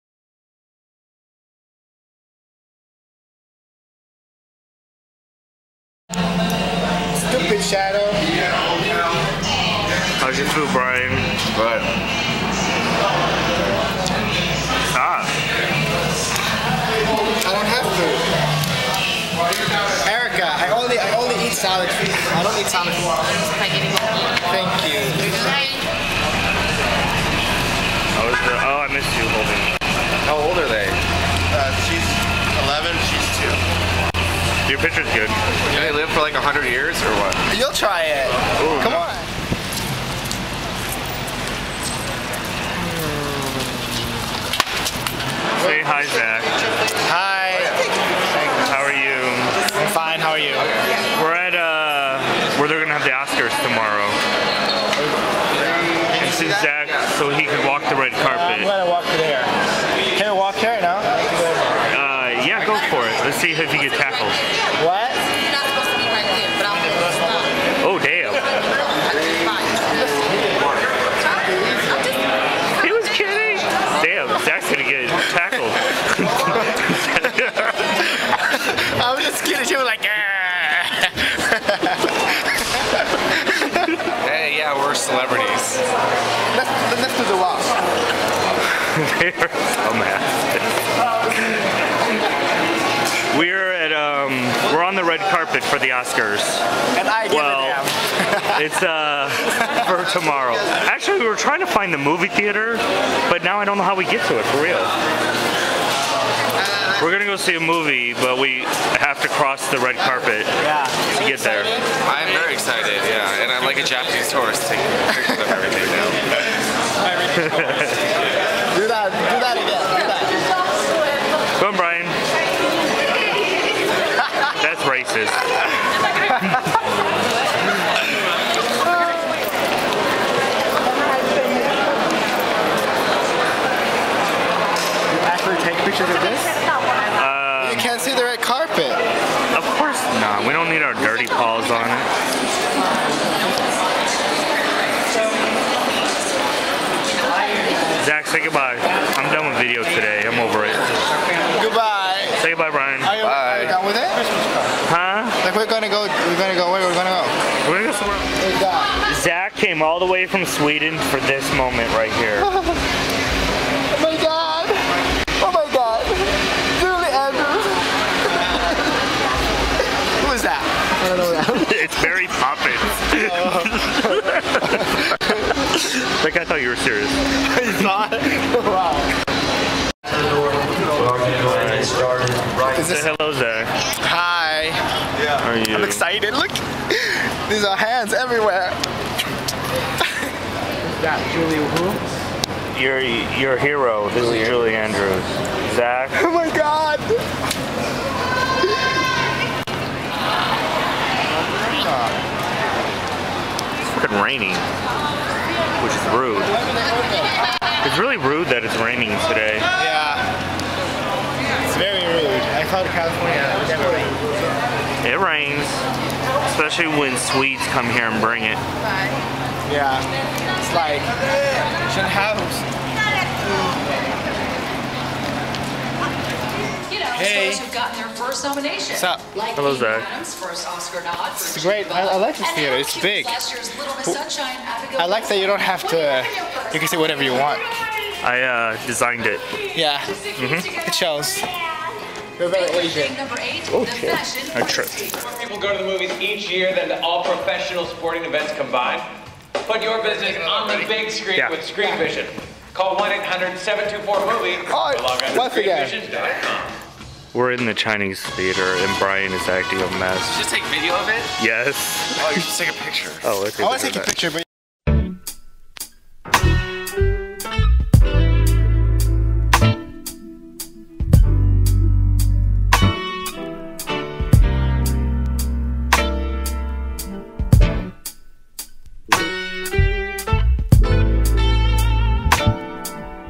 Going to the Movies
Brian, Zack, and I walked down the street to watch the movie, Jumpers. Here is the video of the shit we did on the way...

Jumpers was ok... a for sure DVD movie.